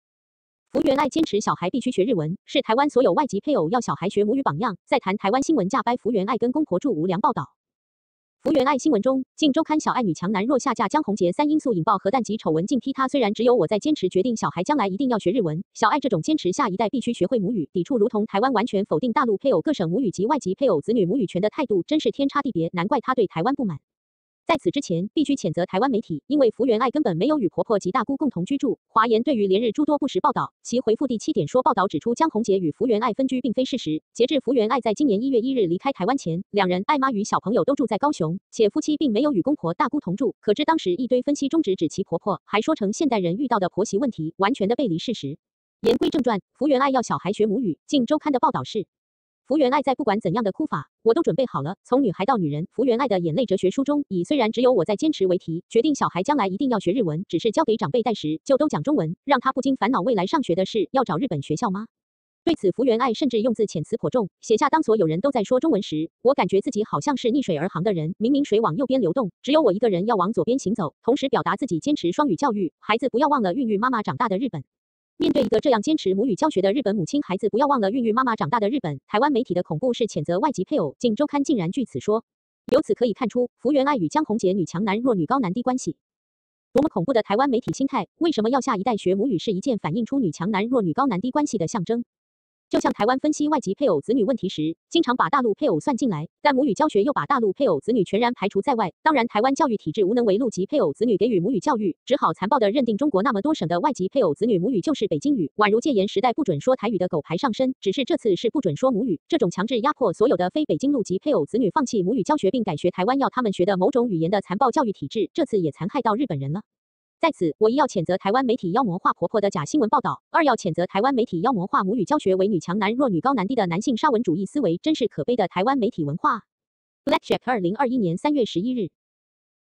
福原愛堅持小孩必須學日文，是臺灣所有外籍配偶要小孩學母語榜樣！再談臺灣新聞假掰福原愛跟公婆住無良報導
福原愛新聞中，鏡週刊小愛女強男弱下嫁江宏傑3因素引爆核彈級醜聞竟批她「雖然只有我在堅持」決定小孩將來一定要學日文！小愛這種堅持下一代必須學會母語，抵觸如同臺灣完全否定大陸配偶各省母語及外籍配偶子女「母語權」的態度，真是天差地別，難怪她對臺灣不滿。

在此之前，必須譴責臺灣媒體，因為福原愛根本沒有與婆婆及大姑共同居住，華研對於連日諸多不實報導，其回覆第7點說「報導指出江宏傑與福原愛分居並非事實，截至福原愛在今年1月1日離開台灣前，兩人、愛媽與小朋友都住在高雄，且夫妻並沒有與公婆、大姑同住。」，可知當時一堆分析中直指其婆婆，還說成「現代人遇到的婆媳問題」，完全的背離事實。

言歸正傳，福原愛要小孩「學母語」，鏡週刊的報導是：

“福原愛在《不管怎樣的哭法，我都準備好了：從女孩到女人，福原愛的眼淚哲學》書中，以「雖然只有我在堅持」為題，決定小孩將來一定要學日文，只是交給長輩帶時，就都講中文，讓她不禁煩惱未來上學的事，「要找日本學校嗎？」

對此福原愛甚至用字遣詞頗重，寫下「當所有人都在說中文時，我感覺自己好像是逆水而行的人，明明水往右邊流動，只有我一個人要往左邊行走。」同時表達自己堅持雙語教育，「孩子不要忘了孕育媽媽長大的日本。」”

面對一個這樣堅持母語教學的日本母親「孩子不要忘了孕育媽媽長大的日本。」，臺灣媒體的恐怖是譴責外籍配偶，鏡週刊竟然據此說：

“由此可以看出，福原愛與江宏傑「女強男弱」「女高男低」關係…”

多麼恐怖的臺灣媒體心態！為什麼要下一代「學母語」是一件反映出「女強男弱」「女高男低」關係的象徵？？

就像臺灣分析外籍配偶子女問題時，經常把大陸配偶算進來，但母語教學又把大陸配偶子女全然排除在外，當然臺灣教育體制無能為陸籍配偶子女給予母語教育，只好殘暴的認定中國那麼多省的外籍配偶子女「母語就是北京語」，宛如戒嚴時代「不准說台語」的狗牌上身，只是這次是「不准說母語」！這種強制壓迫所有的非北京陸籍配偶子女「放棄母語教學」並「改學臺灣要他們學的某種語言」的殘暴教育體制，這次也殘害到日本人了！

在此，我一要譴責臺灣媒體「妖魔化婆婆」的假新聞報導，二要譴責臺灣媒體「妖魔化母語教學」為「女強男弱」「女高男低」的男性沙文主義思維，真是可悲的臺灣媒體文化啊！

Blackjack 2021/3/11